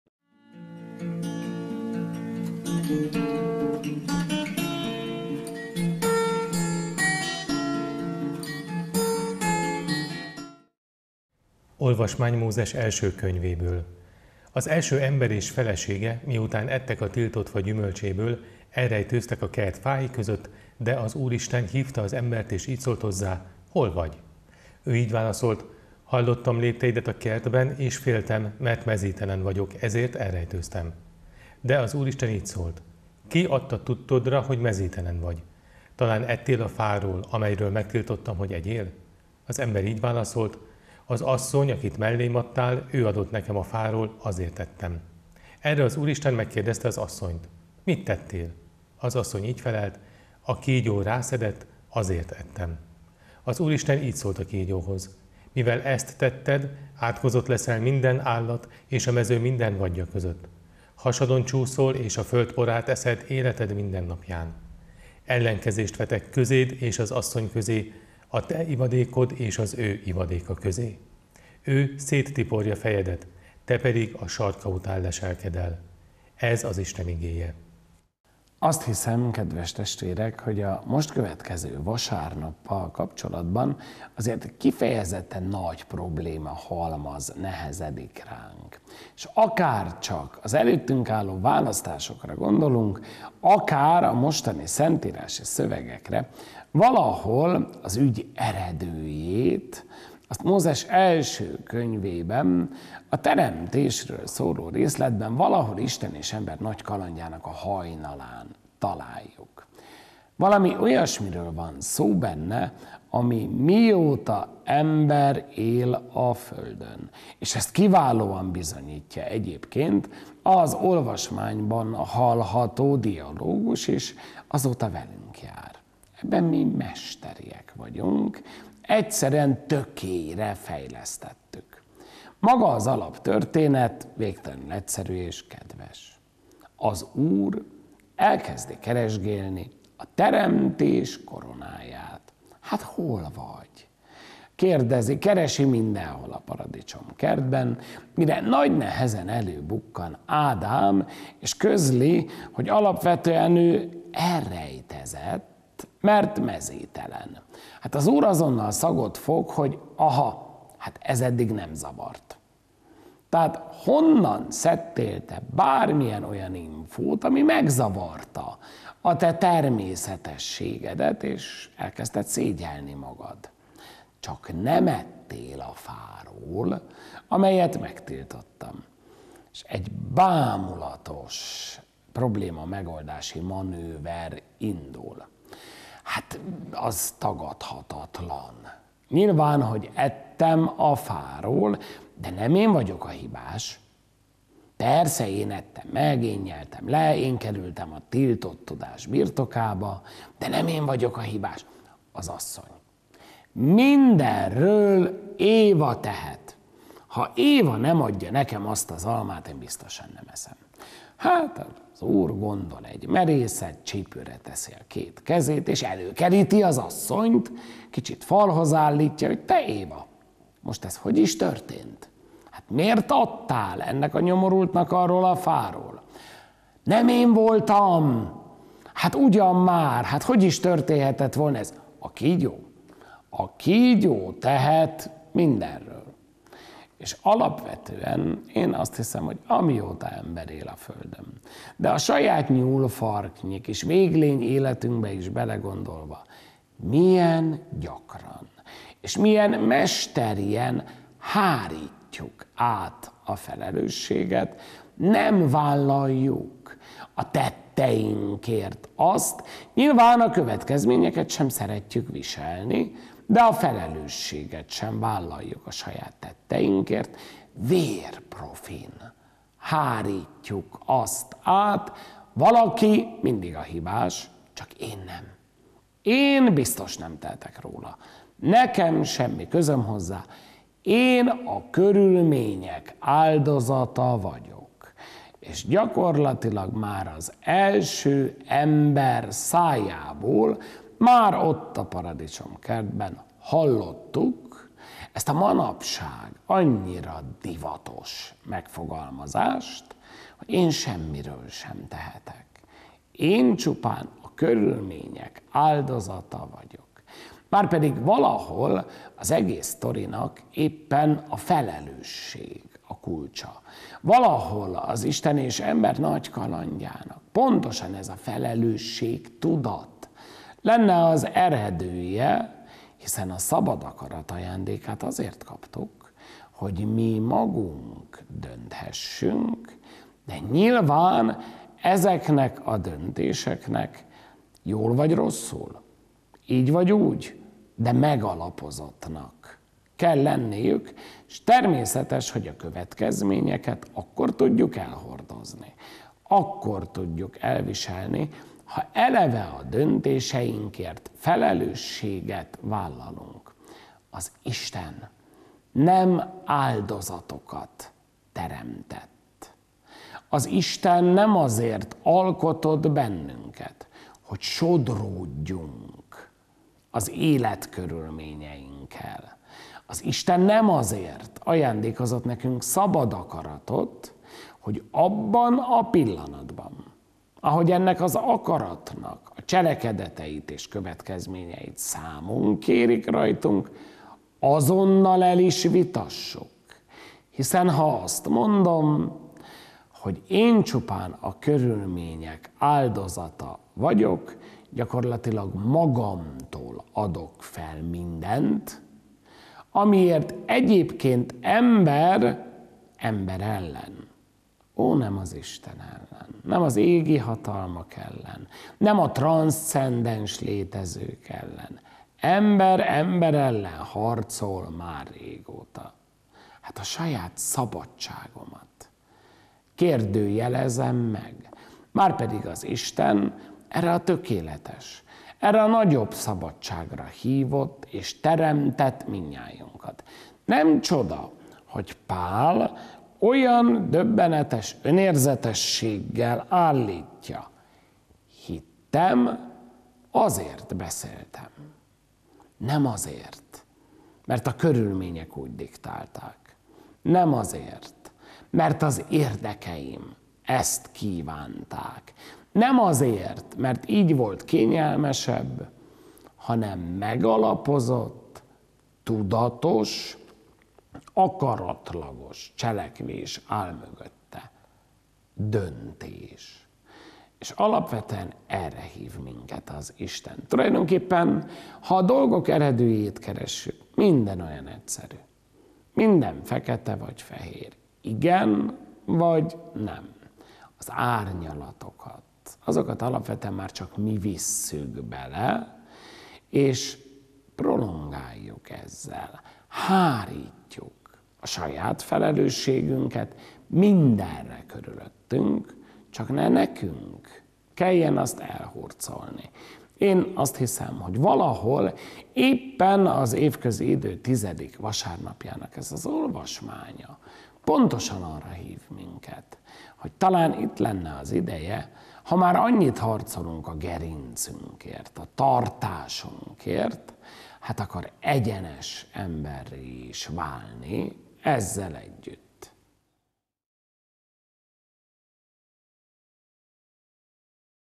Vasárnapi Ige - Évközi 10. vasárnap
Heiter Robert Gottfried szepetneki plébános gondolatai